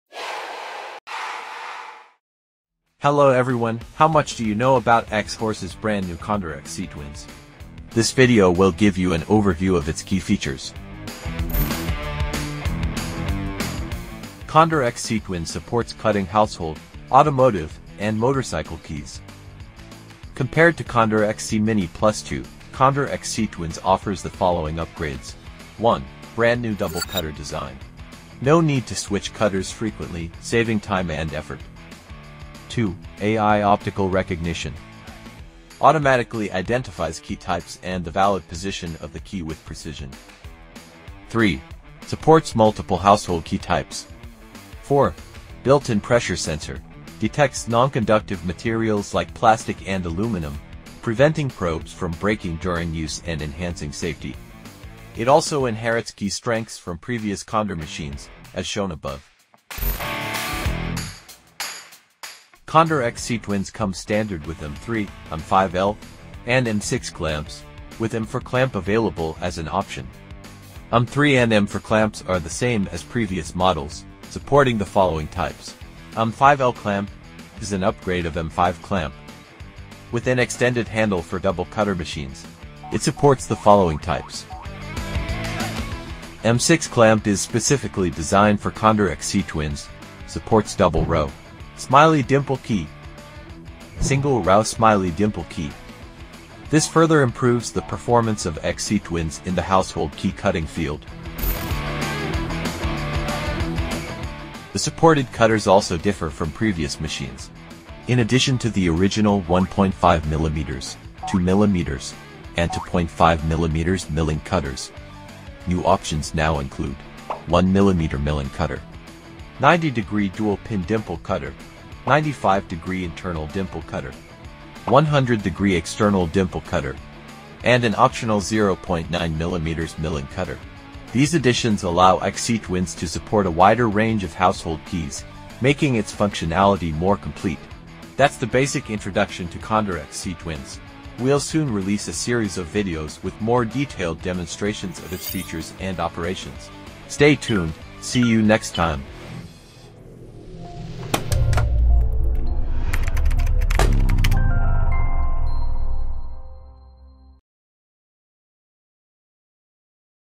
How to use Xhorse CONDOR XC-TWINS Global Launch
This is the new Xhorse XCMT01GL, a new double-cutter intelligent key cutting machine.

Features:

AI Optical Recognition: Equipped with a camera that automatically identifies key types and the valid position of the key, supports adjusting key blank sizes.
Newly Double Cutter Design: Supports cutting various keys (especially residential keys) without frequent cutter changes.
Brand-New Pressure Sensor Technology: Equipped with a built-in pressure sensor at the probe side, which helps avoid probe breaking. Effortlessly identifies aluminum keys.
All-Purpose Clamps: Various functional clamps (M5L, M6, M3, M4) cover a wide range of needs, supporting key duplication for cars, residential use, motorbikes, etc.
Customize Key Type: Supports customization of key bitting data and key blank shapes.
7" Display Intelligent Touch Screen: Equipped with a 7-inch, angle-adjustable screen, making work easier.
Intelligent Color Synchronization: Smart indicator lights automatically change color with the real-time working status.